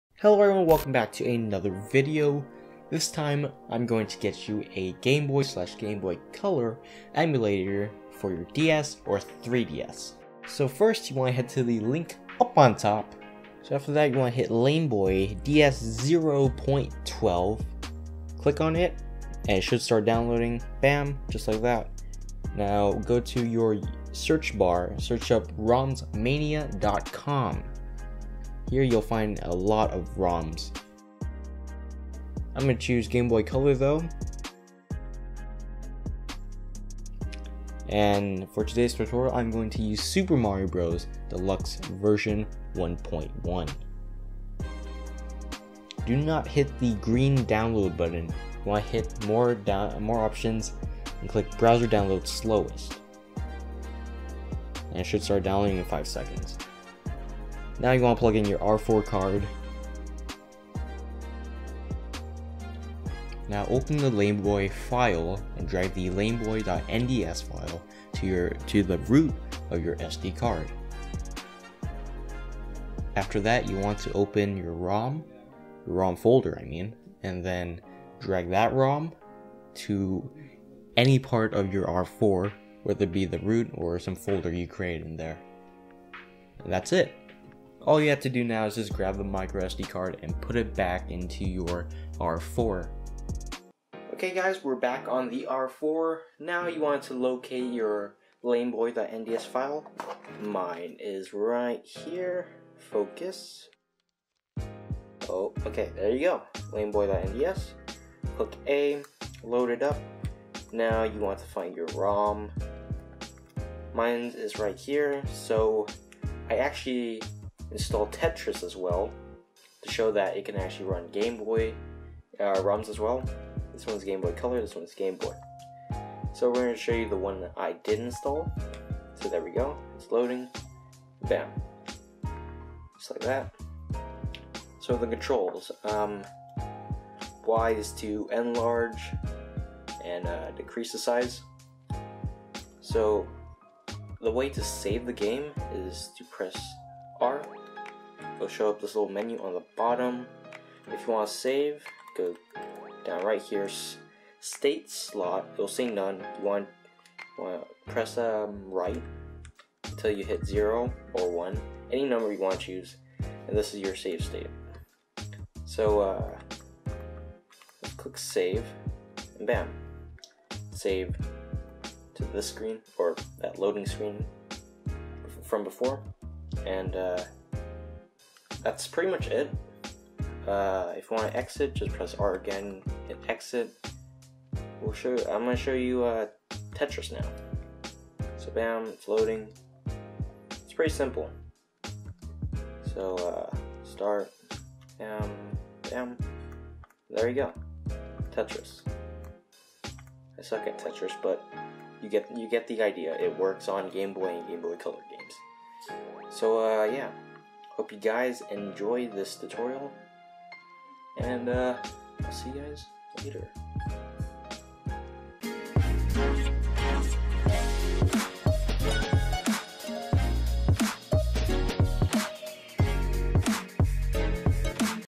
How to get a GB/GBC Emulator for R4 DS/3DS
*Keep in mind that you can use any other rom site to get your roms*